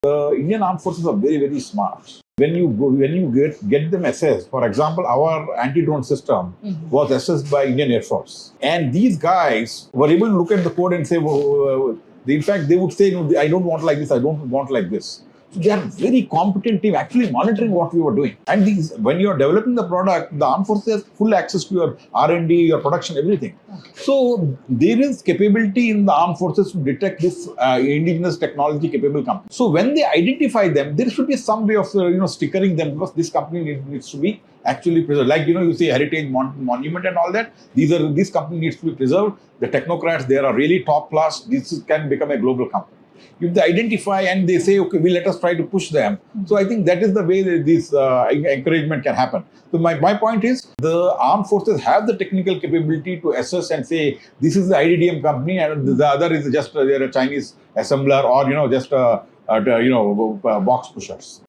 Empowering Indigenous Tech Indian Armed Forces' Role
Zen Technologies has been quietly building some of the most advanced defense training systems and AI-powered solutions, and in this conversation, Ashok breaks down how they’re helping reshape global defense strategies.